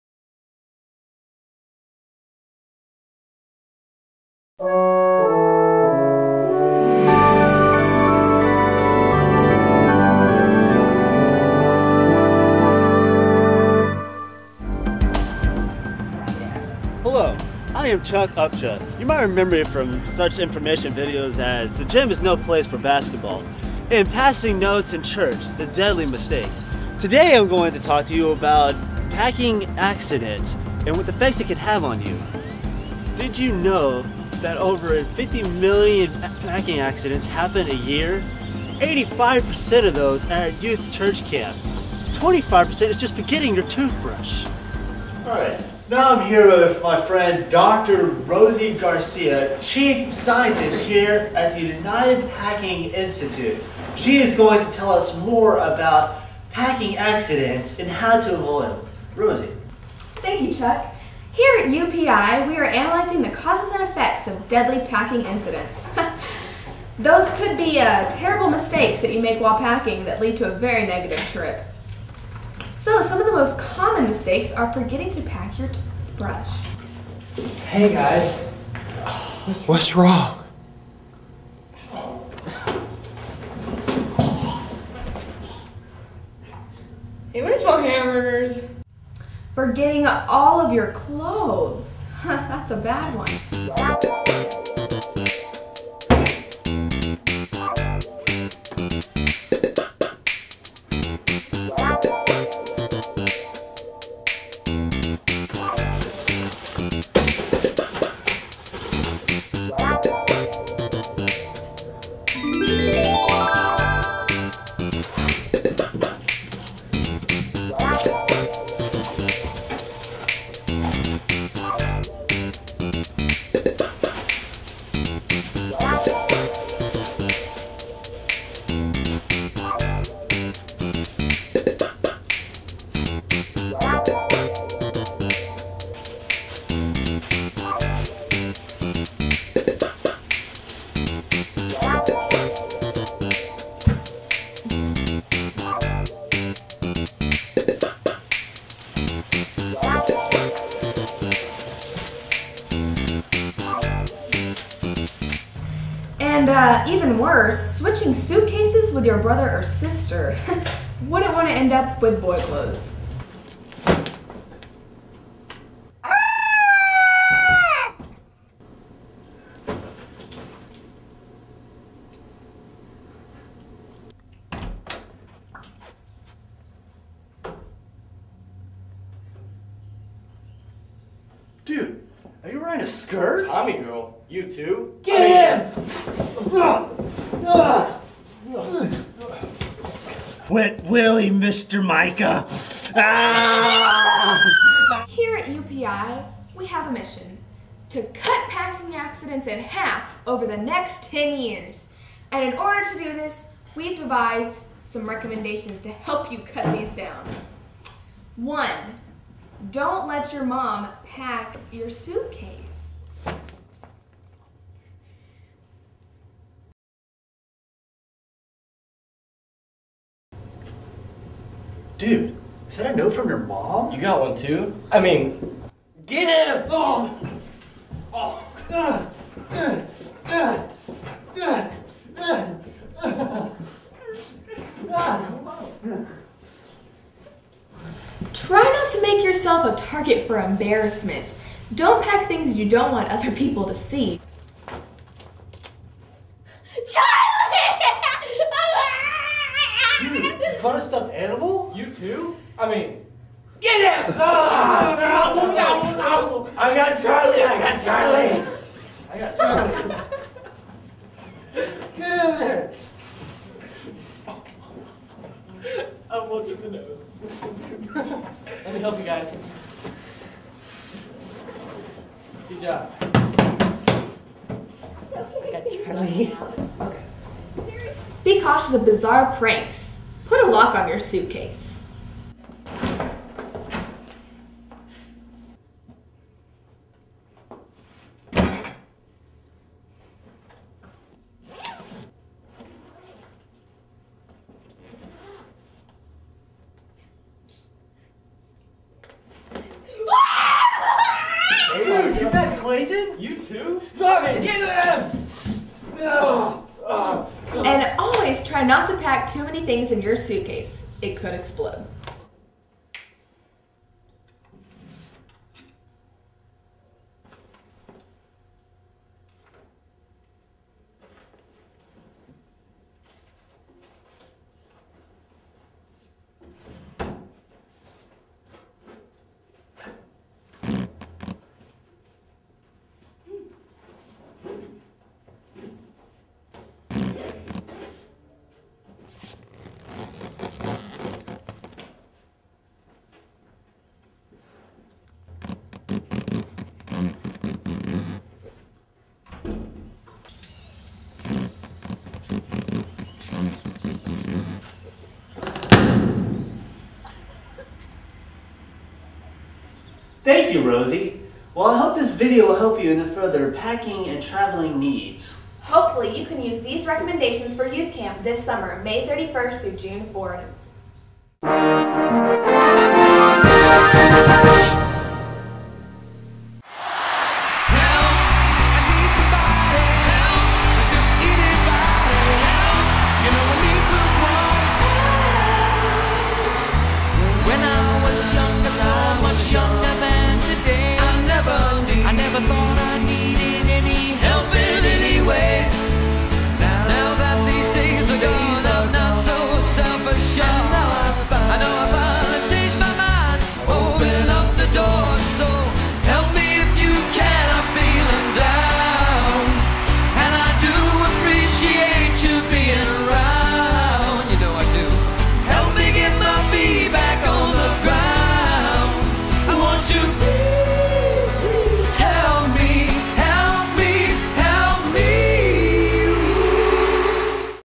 Youth Camp Promo 2004
An old video from New Hope FBC in Cedar Park, TX.  I had some requests to put these videos up, so here they are!